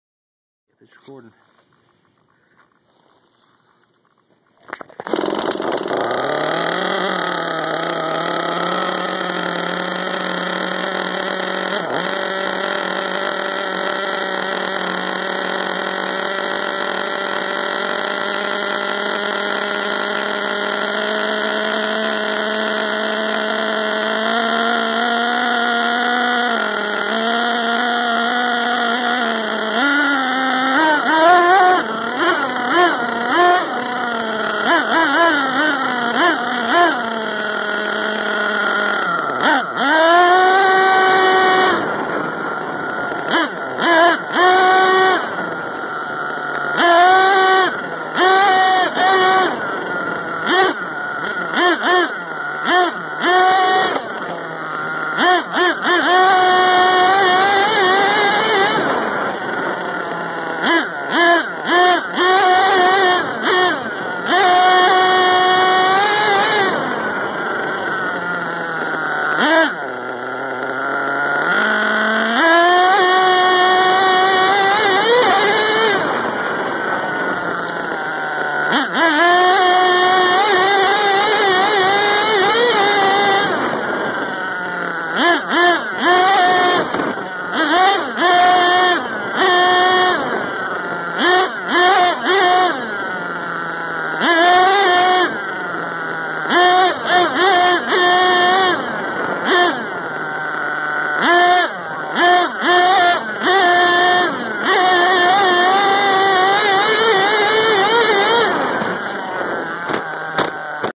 Gas powered rc truck "on-board"
Me and poss strapped my droid x to his monster truck ... Plz rate and ill upload some sick air videos